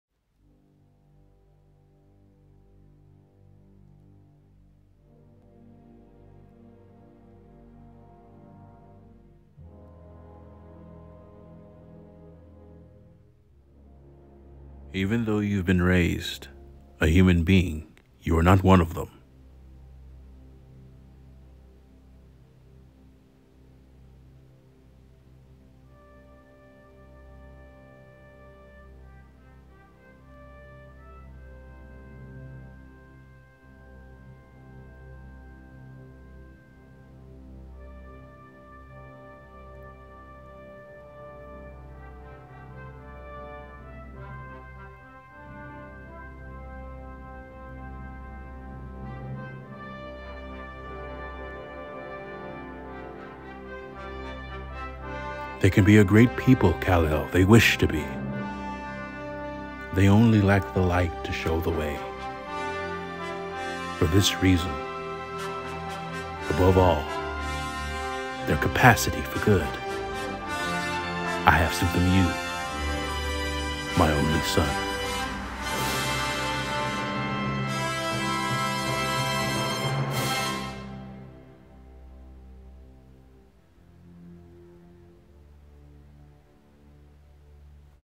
Superman: Man of Steel (Marlon Brando V.O.)
This is something really small. However, I really had fun with this one. I’m a big nerd and enjoy the D.C. and Marvel universes. So naturally I couldn’t help but do the classic Marlon Brando voice over for the character of Jor-El.